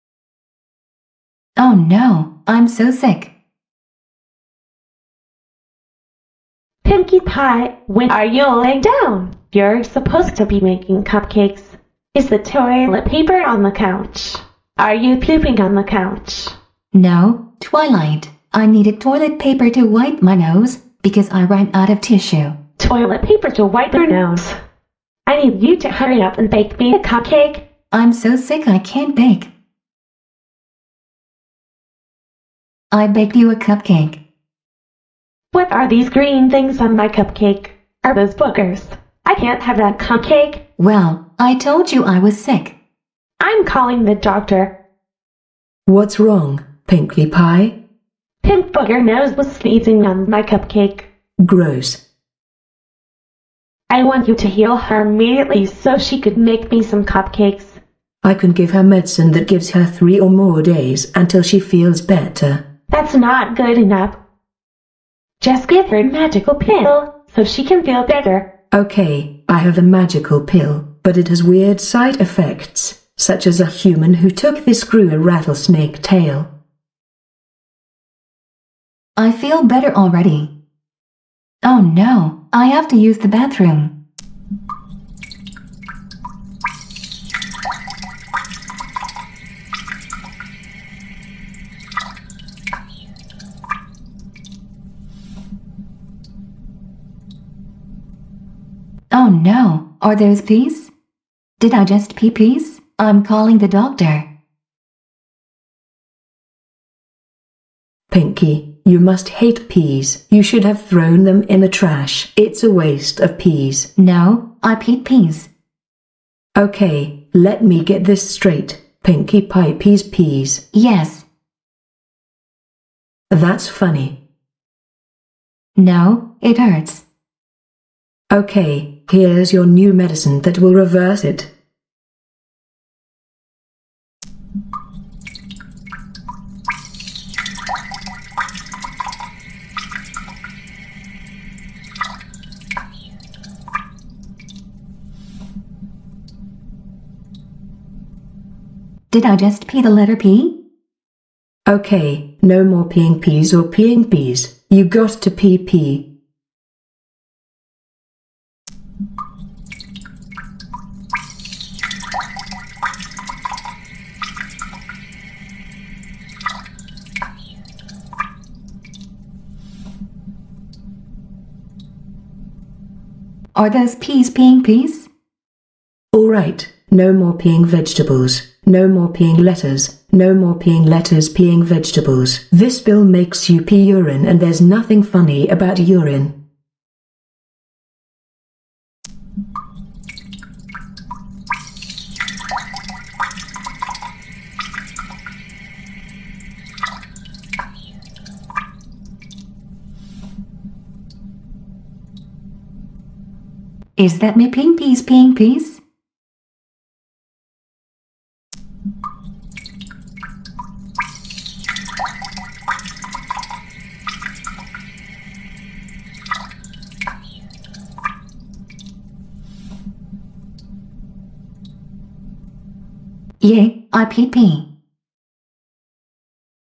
Pony Movie: Pinkie Pie Pees Peas
Pinkie Pie is really sick.

Pony Question: What would you do if you peed peas?